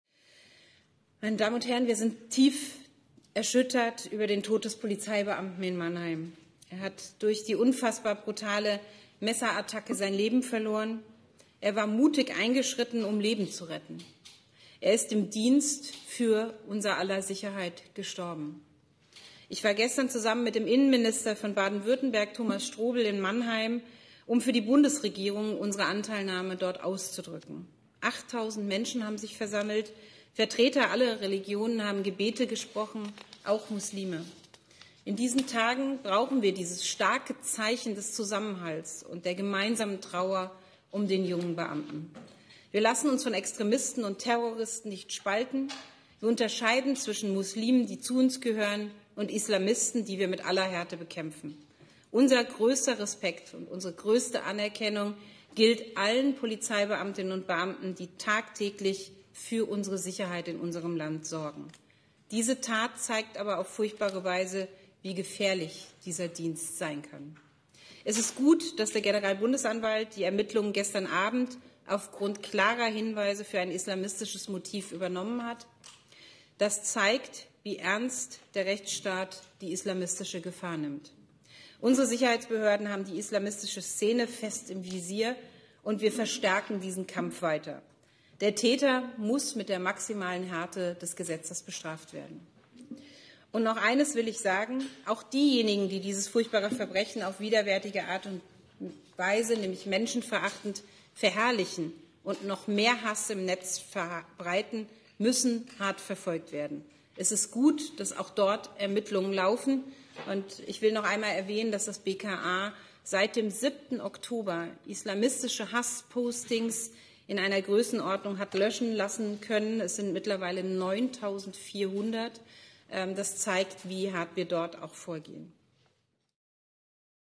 Faeser zu Mannheim: Wir haben die Islamisten im Visier | AFP
Bundesinnenministerin Nancy Faeser (SPD) hat angesichts des Todes eines Polizeibeamten durch einen mutmaßlichen Islamisten in Mannheim den Einsatz der Sicherheitskräfte gewürdigt. Die Behörden hätten die Islamisten im Visier, versicherte sie in Berlin.